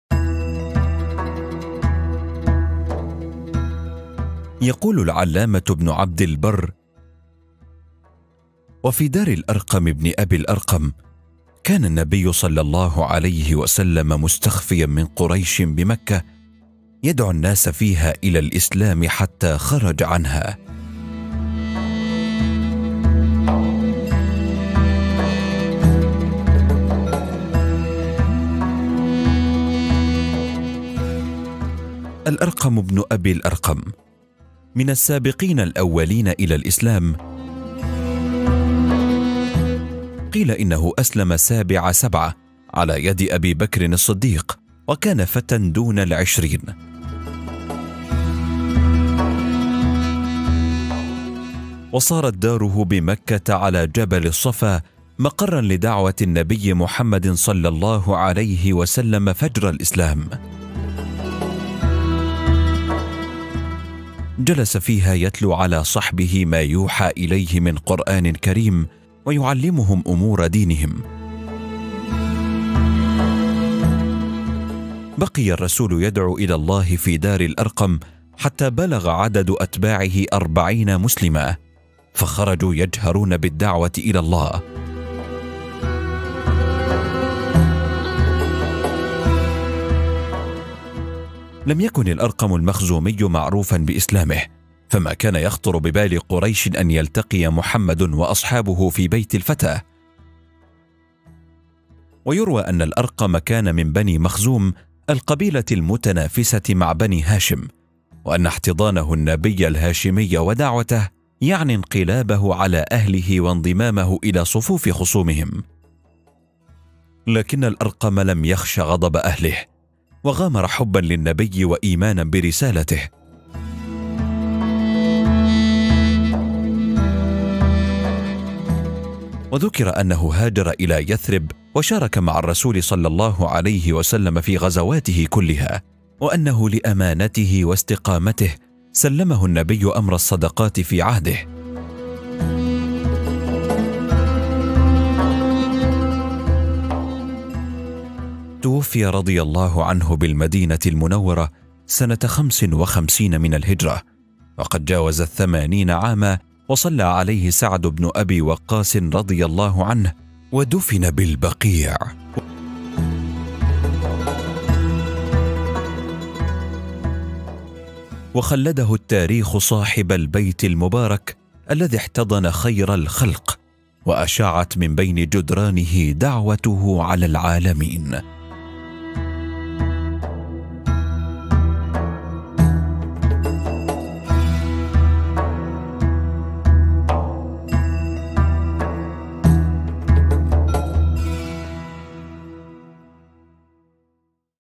دار الأرقم
الأرقم بن أبي الأرقم أسلم قبل أن يبلغ العشرين وكان من السابقين الأولين وهو صاحب أول دار للدعوة الإسلامية
إليكم قصة داره
ـــــــــــــ